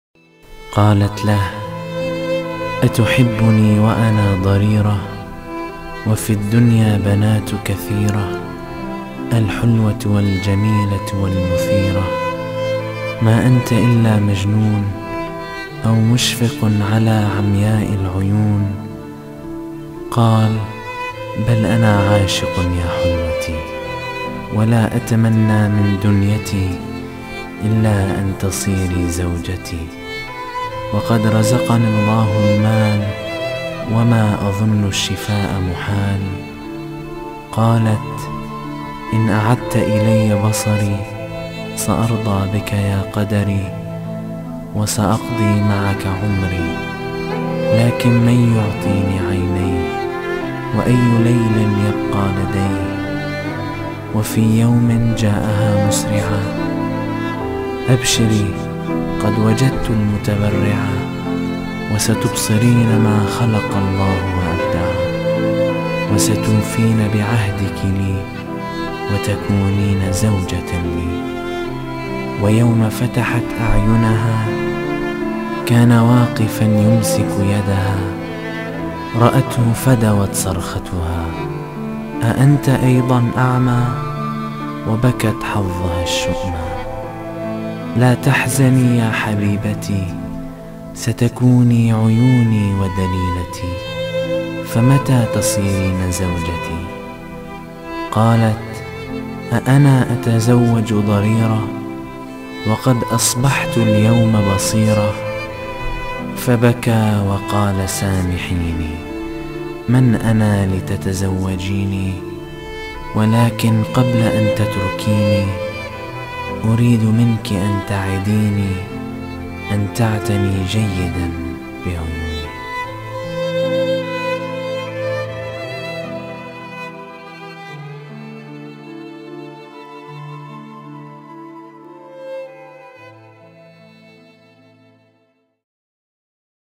Do You Love Me And I Am Blind - Nizar Qabanni [ENGLISH SUBTITLES]
A poem by the late Syrian poet Nizar Qabanni: Do you love me and I am blind.

Self-explanatory. C'est La Vie.